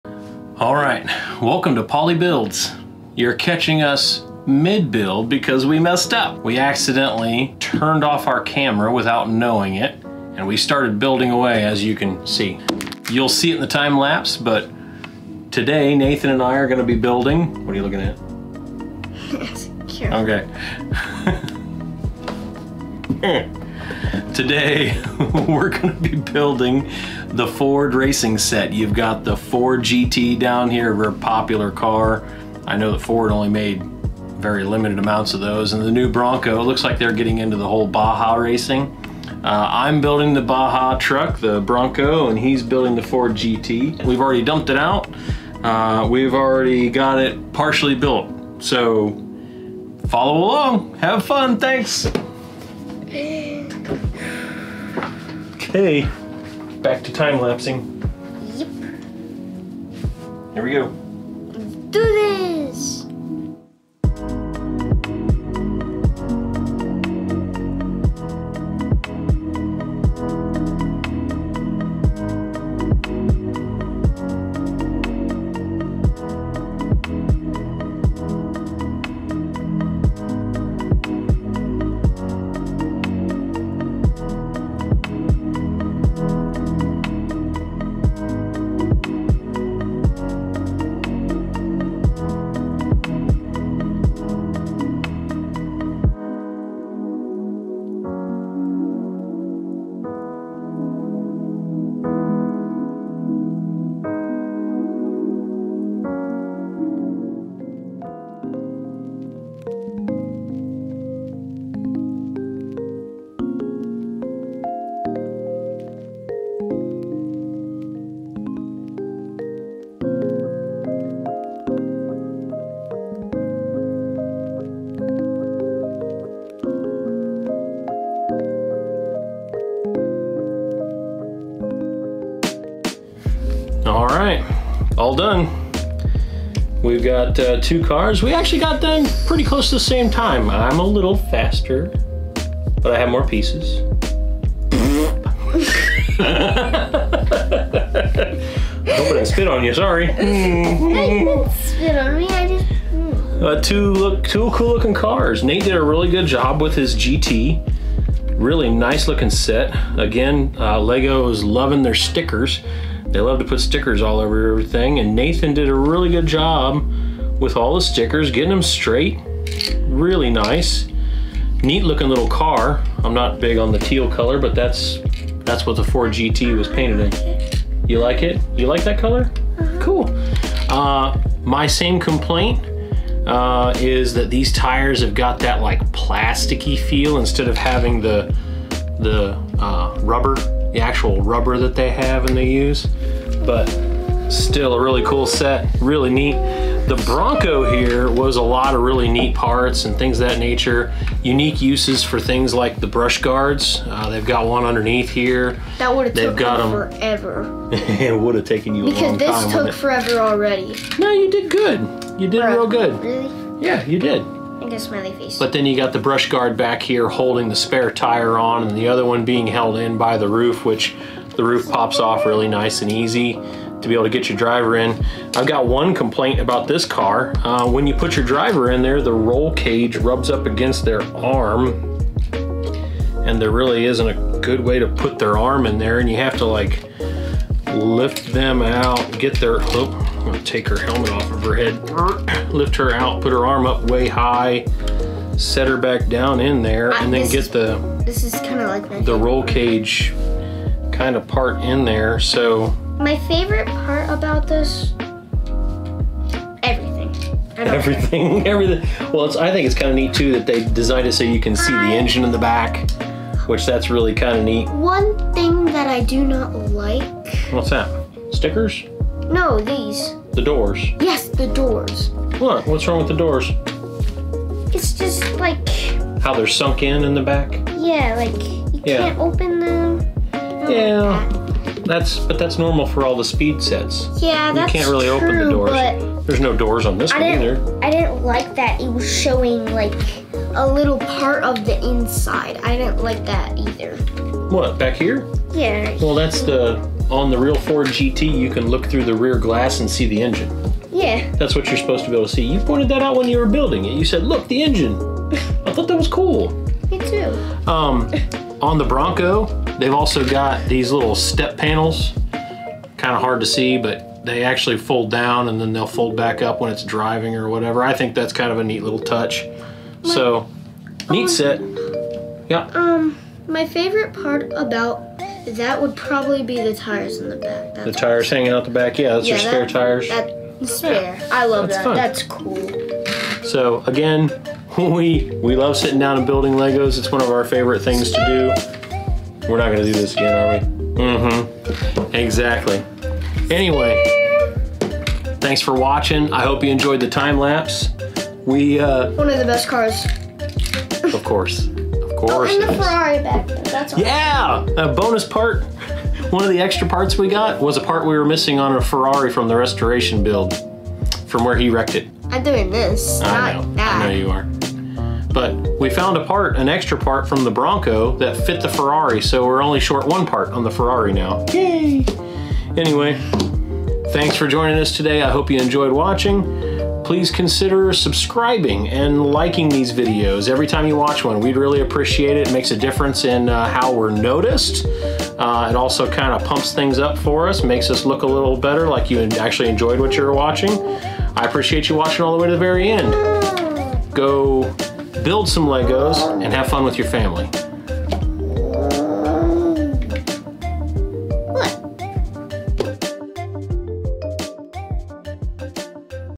Lego# 76905 Ford Speed Champions
Nathan and I really enjoy these Speed sets from Lego.  When we get a chance to do pairs of cars it's always a good time.  We laugh, we joke, we play around and we build some of the coolest looking sets!  Join us as we build the Ford version of Lego Speed!

Music used can be found license free @ Streambeats.com